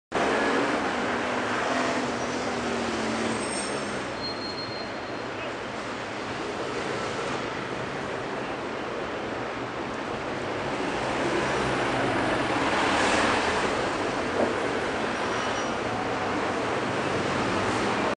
The Incredible Human Machine
Traffic travels down a busy street in London, England.